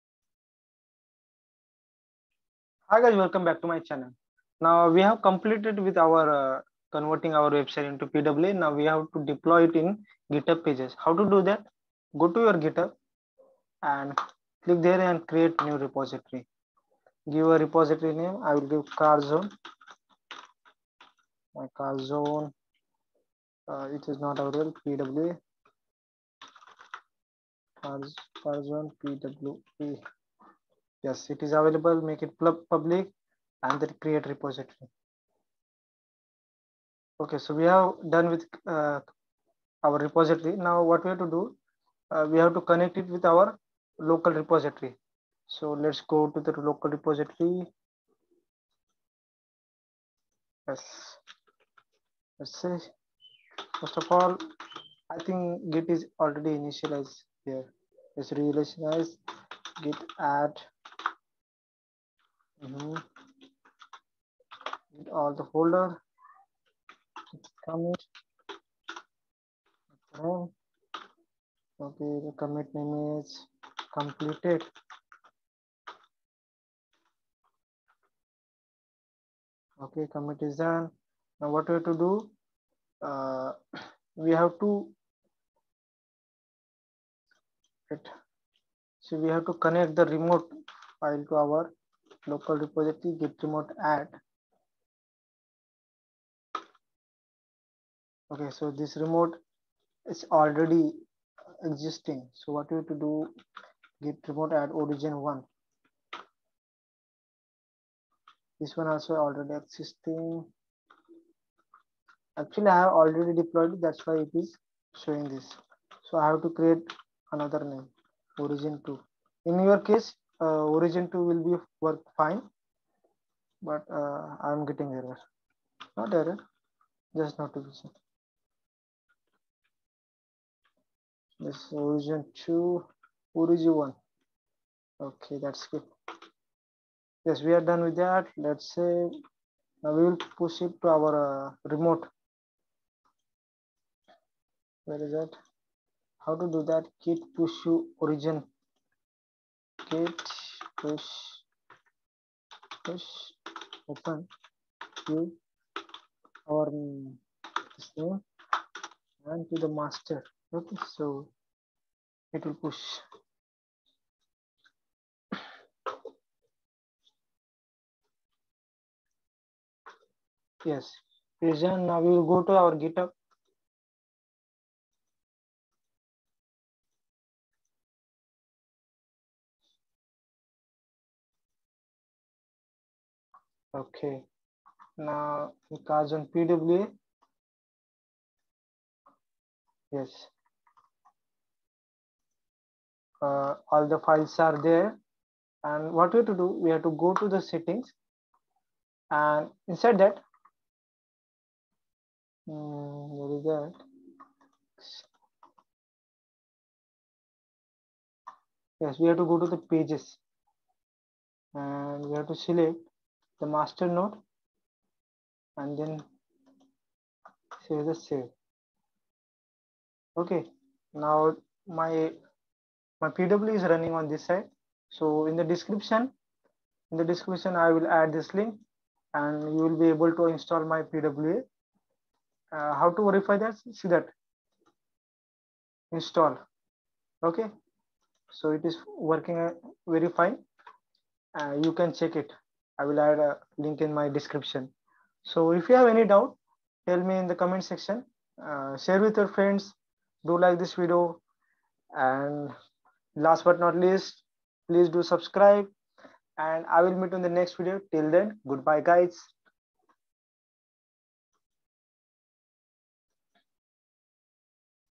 Deploying Progressive web application on GitHub pages | PWA-6
To study and implement deployment of Ecommerce PWA to GitHub Pages

Progressive Web Apps (PWA) deliver a native app-like experience on the web. Learn how to analyze and implement service workers to make your web app installable. Build complete PWAs with me